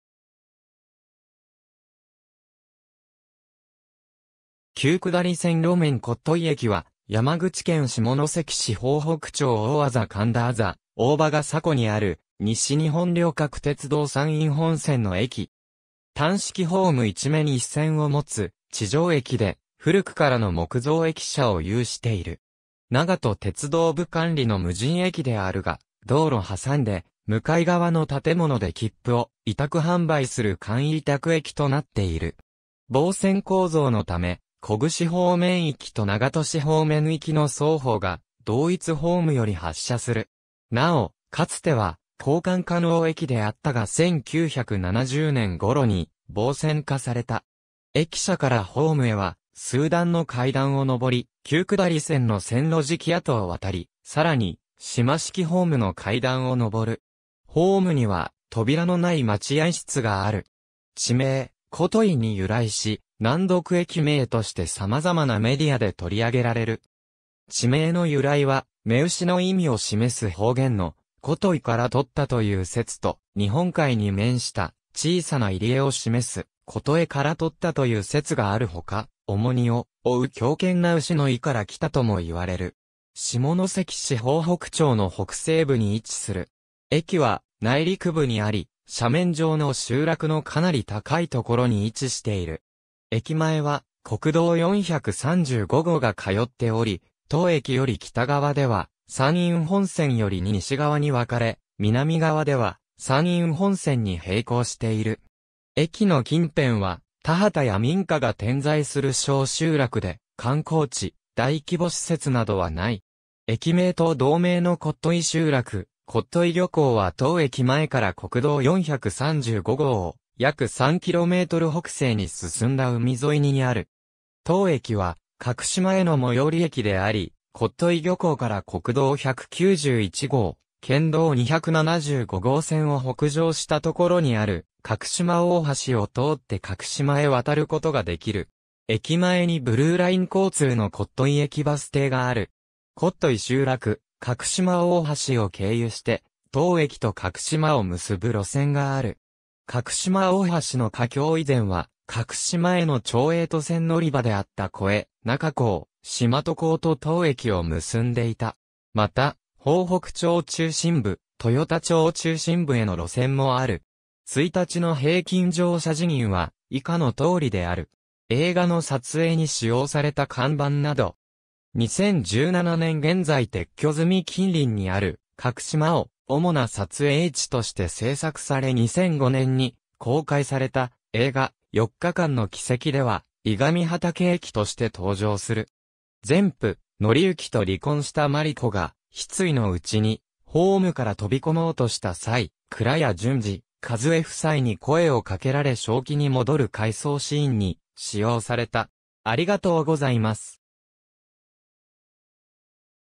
特牛駅
旧下り線路面（待合室付近にホームに上がる階段が見える） 特牛駅（こっといえき）は、山口県下関市豊北町大字神田字大場ヶ迫にある、西日本旅客鉄道（JR西日本）山陰本線の駅。
単式ホーム1面1線（ホームは小串方面に向かって右側に配置）をもつ地上駅（停留所）で、古くからの木造駅舎を有している。
長門鉄道部管理の無人駅であるが、道路挟んで向かい側の建物で切符（常備券のみ）を委託販売する簡易委託駅となっている。
棒線構造のため、小串方面行きと長門市方面行きの双方が同一ホームより発車する。
なお、かつては交換可能駅であったが1970年ごろに棒線化された。
駅舎からホームへは数段の階段を登り、旧下り線の線路敷跡を渡り、さらに島式ホームの階段を上る。
ホームには扉の無い待合室がある。
地名「コトイ」に由来し、難読駅名としてさまざまなメディアで取り上げられる。
地名の由来は、牝牛の意味を示す方言の「コトイ」から取ったという説と、日本海に面した小さな入り江を示す「琴江」から取ったという説があるほか、「重荷を負う強健な牛」の意から来たともいわれる。
下関市豊北町の北西部に位置する。
駅は内陸部にあり、斜面上の集落のかなり高いところに位置している。
駅前は国道435号が通っており、当駅より北側では山陰本線より西側に分かれ、南側では山陰本線に並行している。
駅の近辺は田畑や民家が点在する小集落で、観光地・大規模施設などはない。
駅名と同名の特牛集落（大字神田特牛）・特牛漁港は当駅前から国道435号を約3km北西に進んだ海沿いにある。
当駅は角島への最寄り駅であり、特牛漁港から国道191号（北浦街道）・県道275号線を北上したところにある角島大橋を通って角島へ渡ることができる。
駅前にブルーライン交通の特牛駅バス停がある。
特牛集落・角島大橋を経由して当駅と角島を結ぶ路線がある。
角島大橋の架橋以前は角島への町営渡船乗り場であった肥中港・島戸港と当駅を結んでいた。
また、豊北町中心部・豊田町中心部への路線もある。
1日の平均乗車人員は以下の通りである。
映画の撮影に使用された看板など。
2017年現在撤去済み 近隣にある角島を主な撮影地として制作され2005年に公開された映画『四日間の奇蹟』では、「伊上畑駅」（いがみはたえき）として登場する。
前夫・則幸と離婚した真理子（石田ゆり子）が失意のうちにホームから飛び込もうとした際、倉野順次（西田敏行）・和枝（松坂慶子）夫妻に声をかけられ正気に戻る回想シーンに使用された。